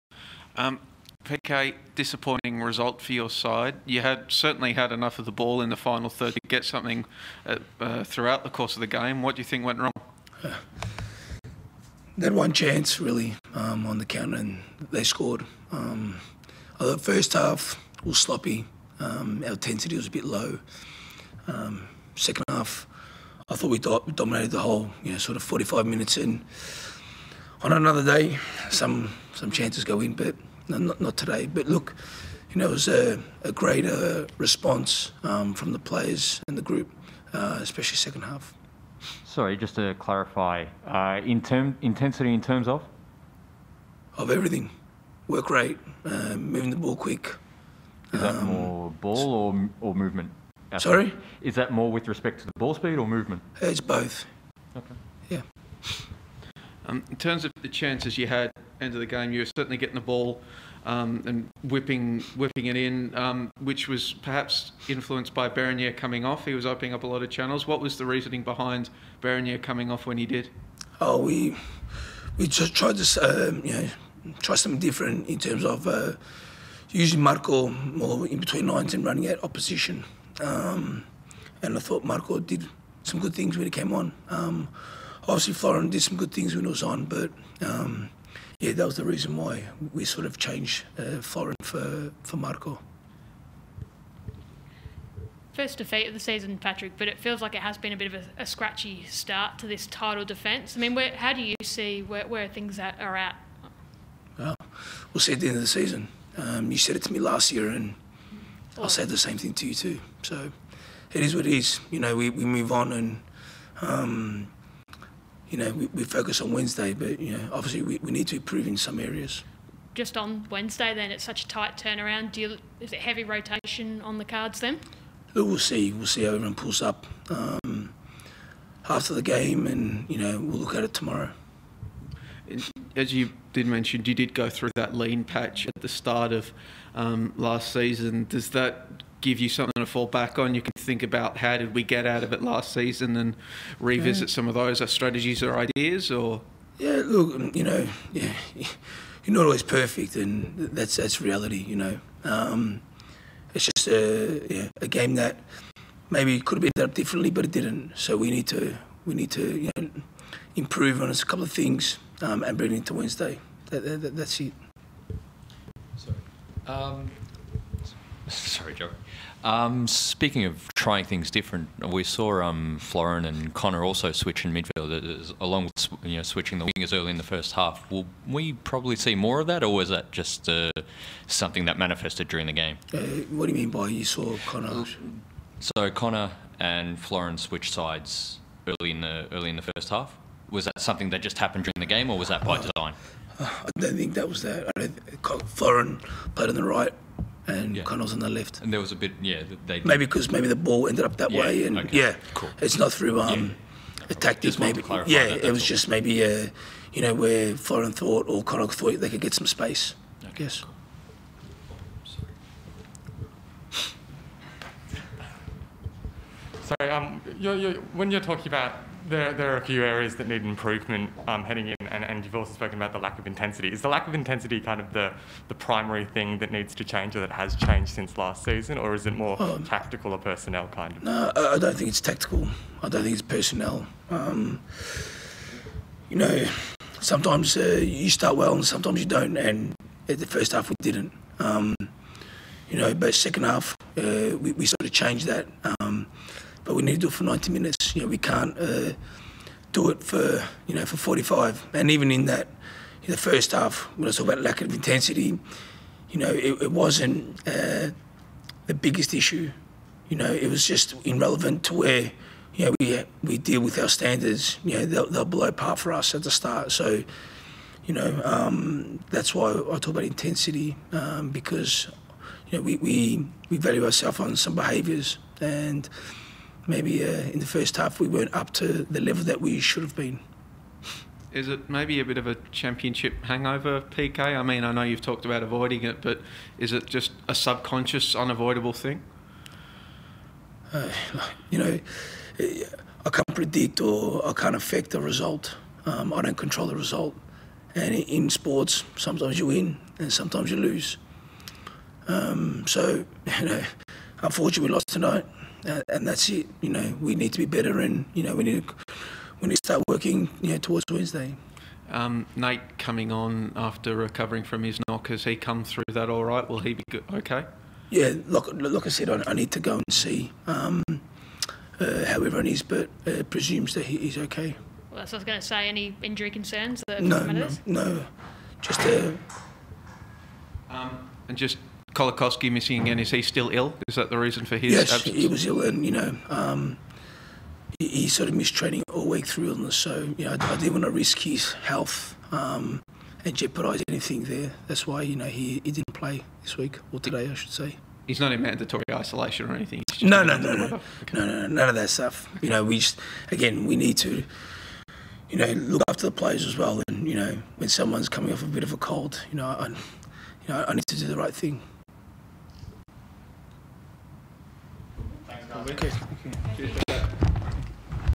Post-match Press Conference: Patrick Kisnorbo | 4/12/21
City Head Coach Patrick Kisnorbo spoke with media following his teams 0-1 loss to Western United.